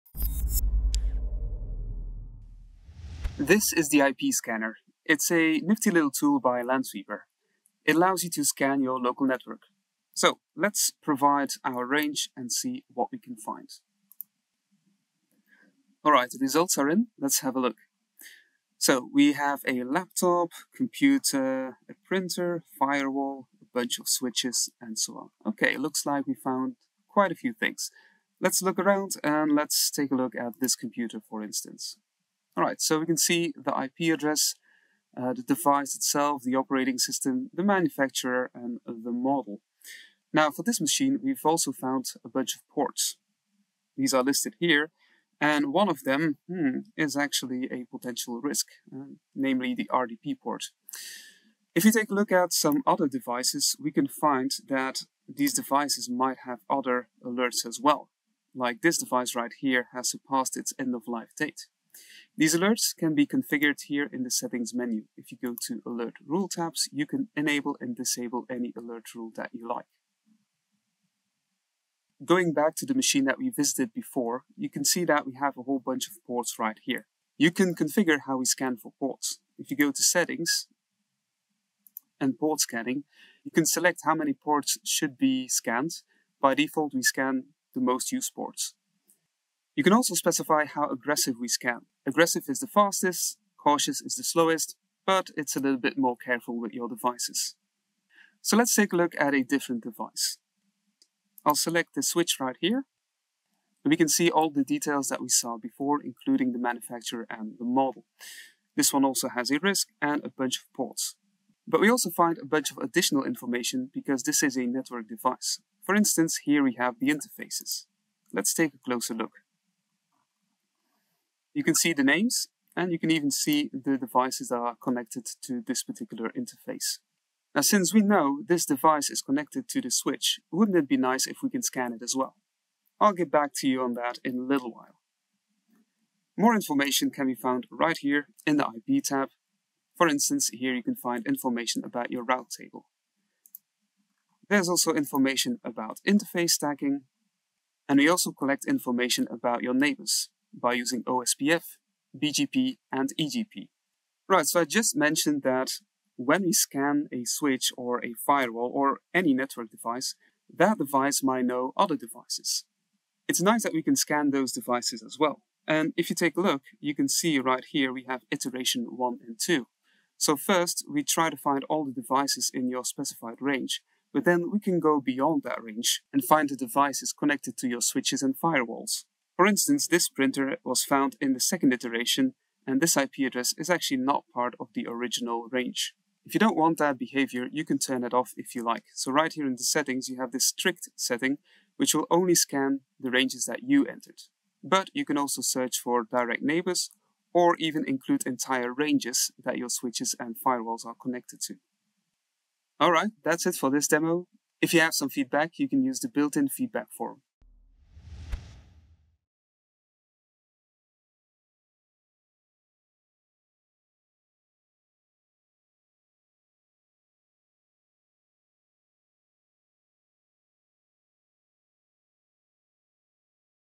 IP Scanner - Free Network Scanning Tool Powered by Lansweeper
Lansweeper’s advanced network scanner is a free IP scanner that can scan IP ranges and find every device connected to your network without the need to install software on them. This is an indispensable tool for network administrators who want to manage their IT Inventory. Download the free IP Range scanning tool

Scan IP Ranges Automatically or on Demand
Re-Discover an Entire Subnet with Just One Click

With Lansweeper’s advanced IP scanner, you can gather relevant network information via WMI, SNMP, HTTP, FTP, SSH, and an array of other protocols.